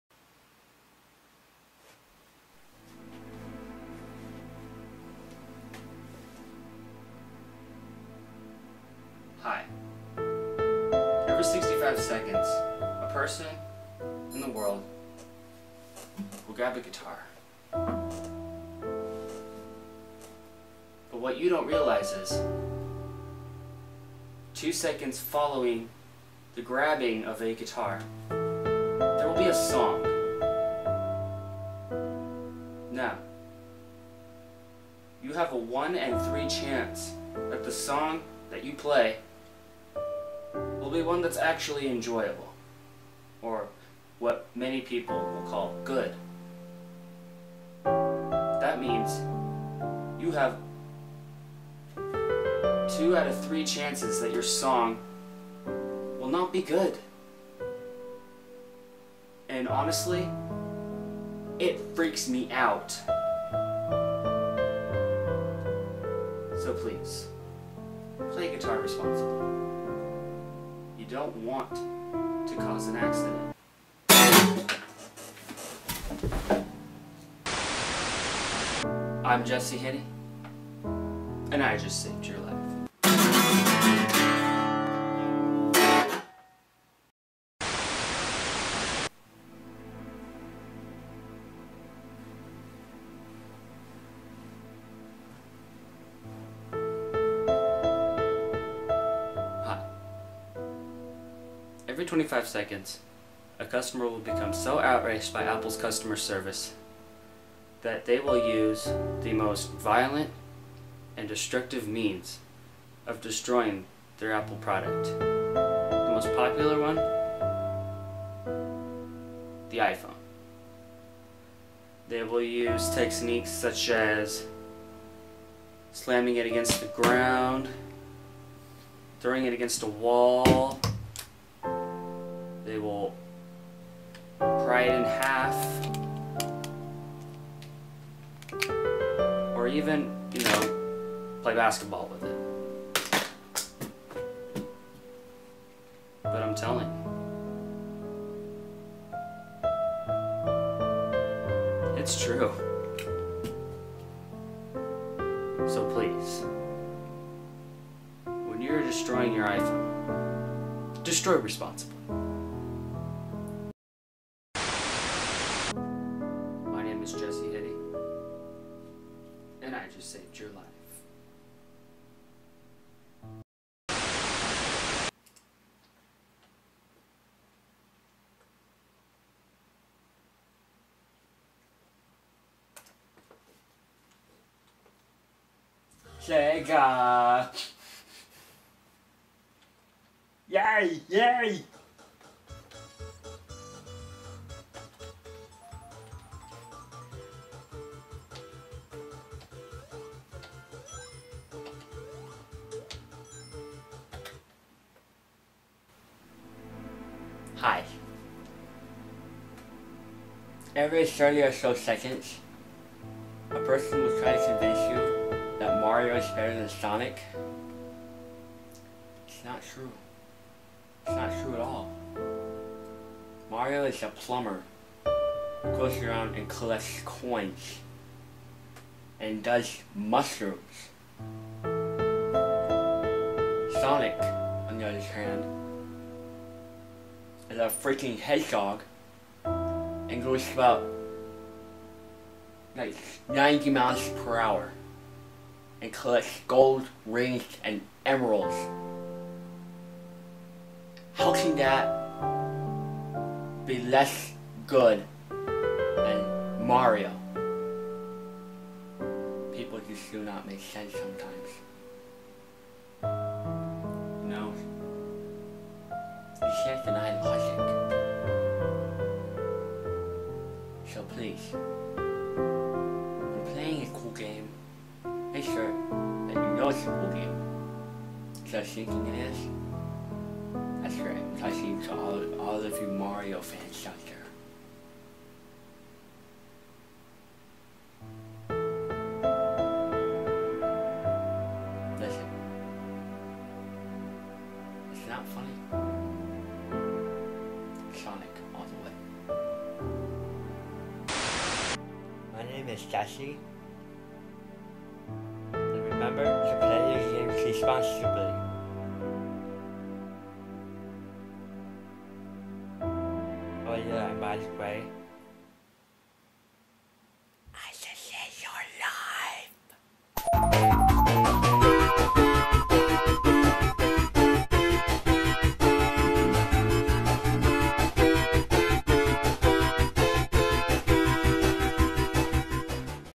PSA 3 Pack!!
Get ready everyone! It's a PSA triple kill! PSA #1 is about the dangers that come with playing guitar and how playing the wrong song can have almost fatal results. PSA #2 reminds us that even though its fun to destroy expensive electronics (such as the iPhone), always destroy responsibly. And PSA #3 is all about the rivalry between video game's top heros, Mario and Sonic, and who truely is worthy of being crowned #1. Boo Yaa!!